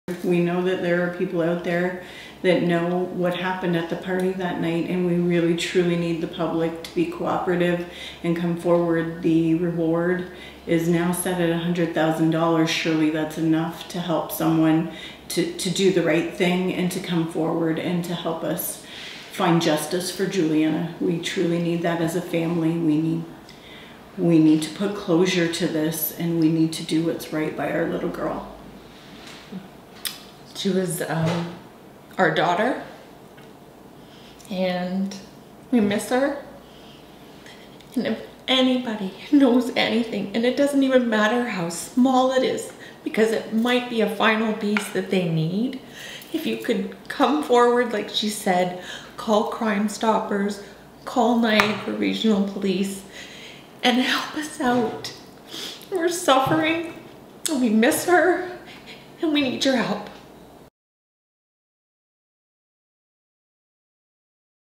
Niagara Regional Police seek suspects in Windsor woman's murder, family pleads for help.
Juliana Pannunzio's step mother Shellie Pannunzio and mother Lisa Mulcaster plead for help in the investigation of her murder on Thursday, July 8, 2021, in Essex. Juliana was murdered in the Niagara area earlier this year and police are increasing reward money for leads into the crime.